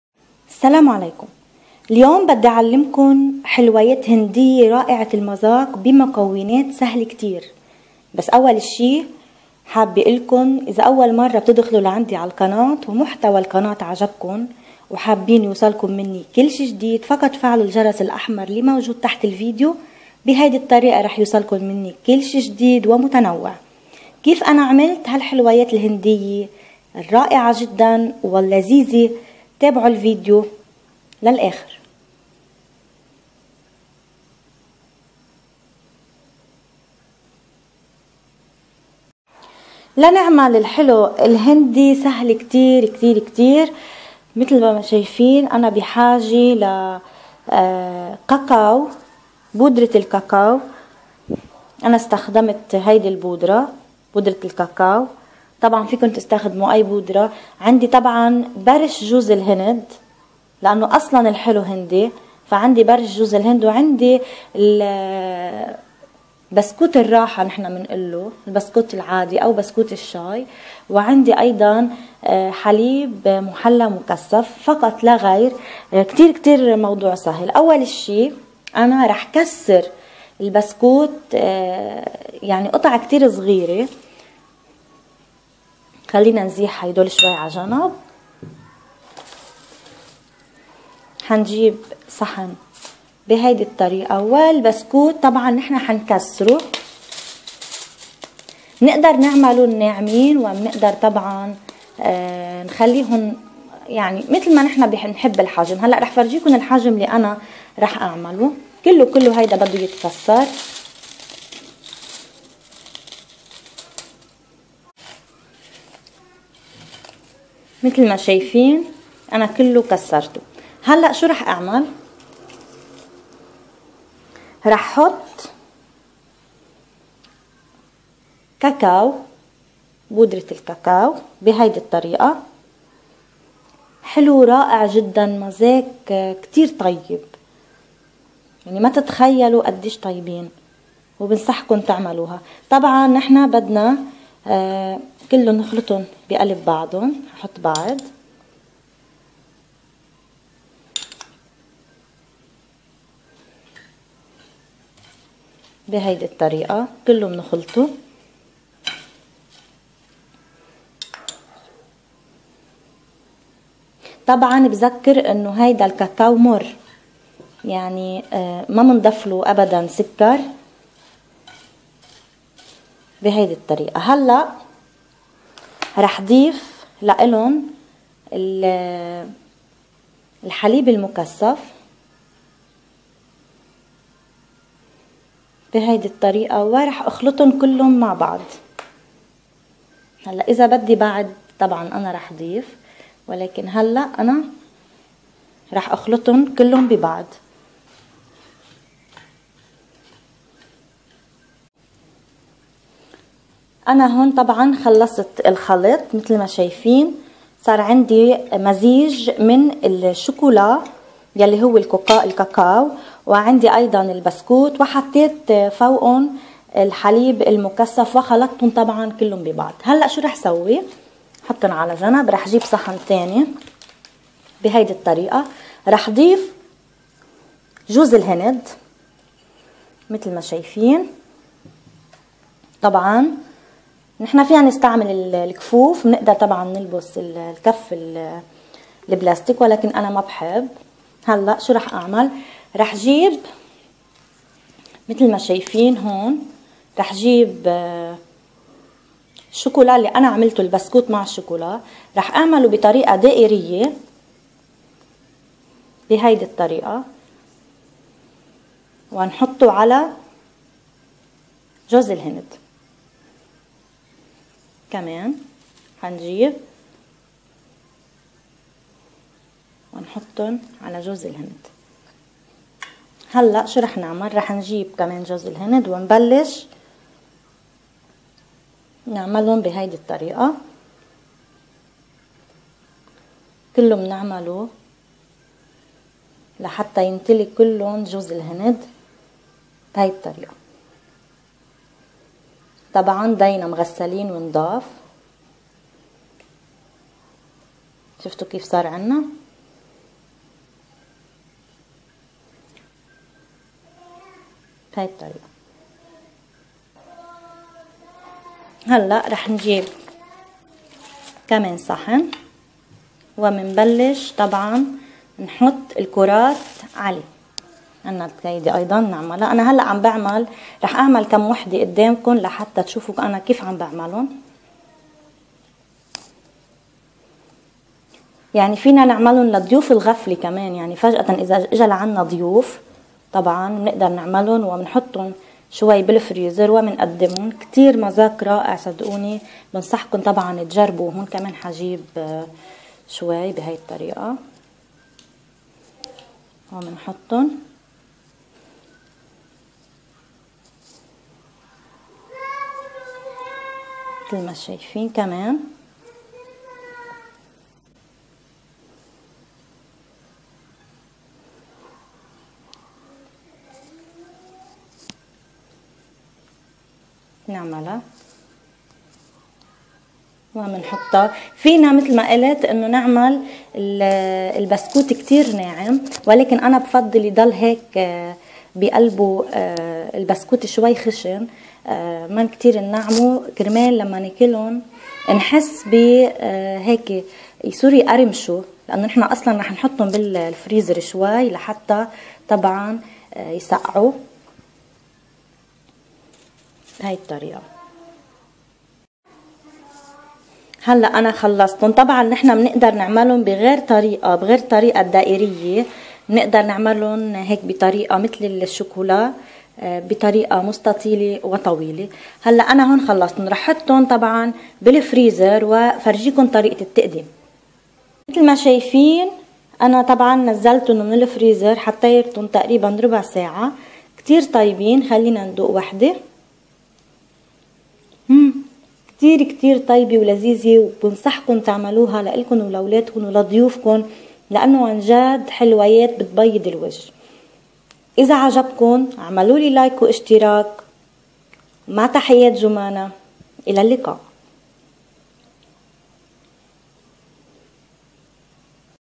حلويات هنديه رائعه المذاق شهيه جدا بمكونات بسيطه وسهله وبخمسة دقائق بتكون جاهزه فقط
الحلو الهندي:
 بسكوت.
حليب مكثف محلى.
كاكو بودره.
برش جوز الهند.

قناتي الثانيه ارجو الاشتراك

توت زمور خطر